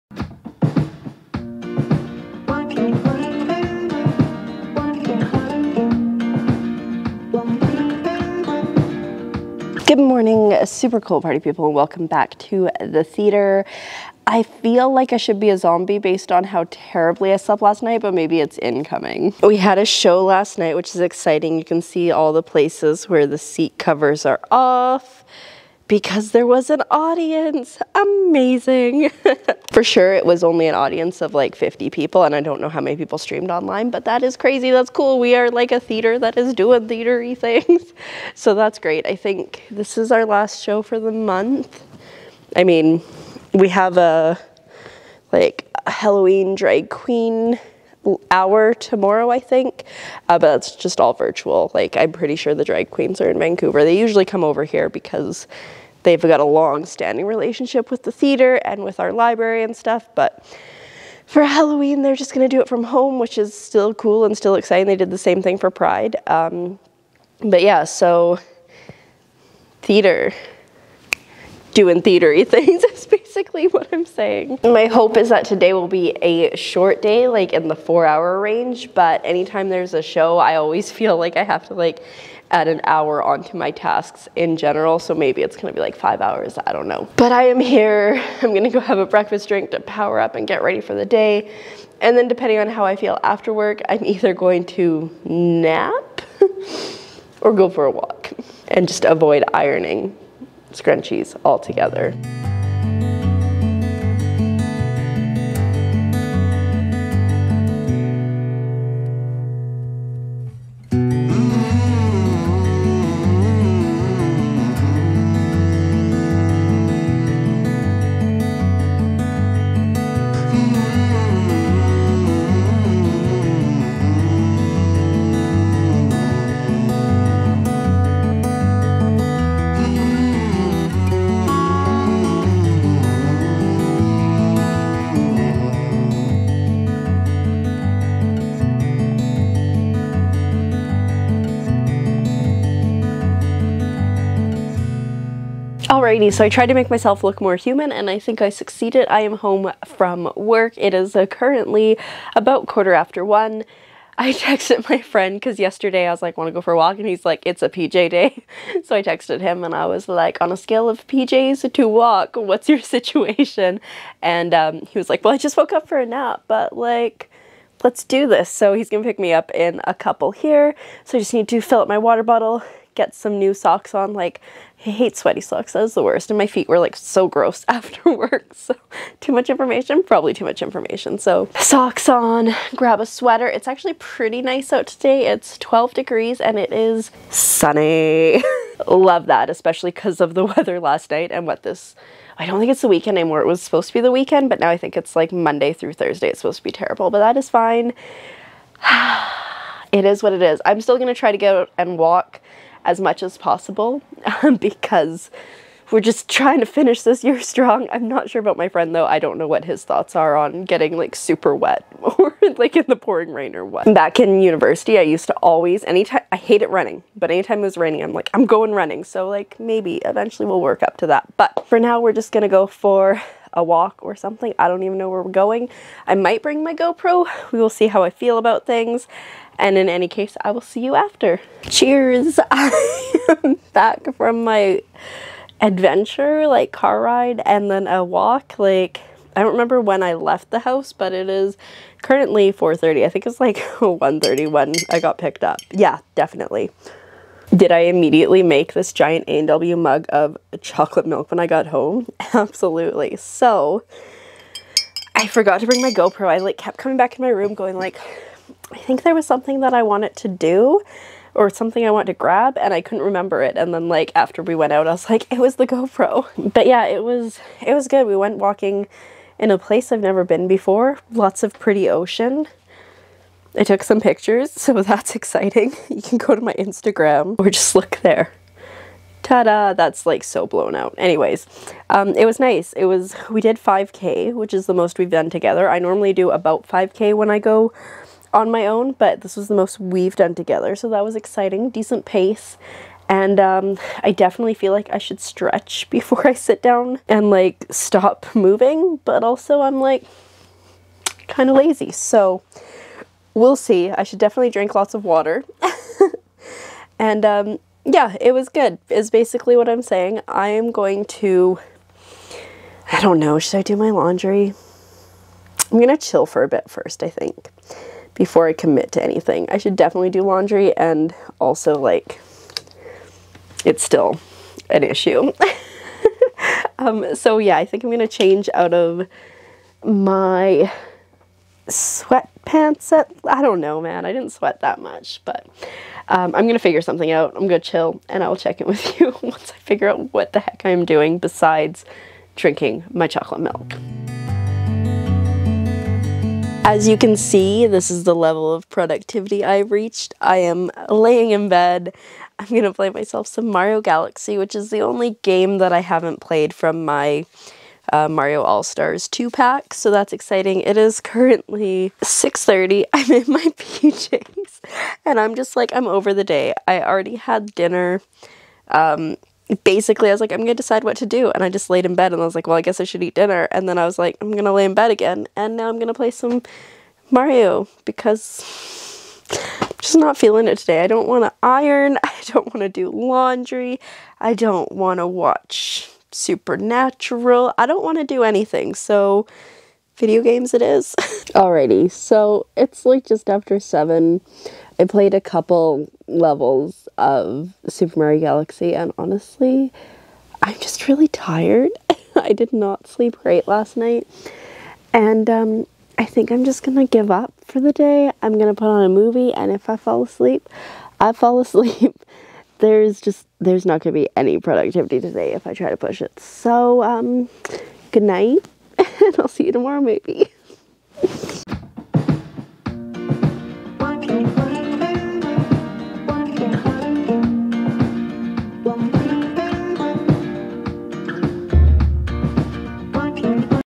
Barely Functioning | October 30, 2020
The struggle is very real to do anything. And so I protested and did almost nothing.

Etsy Store

Equipment:
Camera: Canon EOS M50
Mic: Rode Wireless Go
Tripod: Manfrotto MTPIXI-B PIXI Mini Tripod
Editing Software: Filmora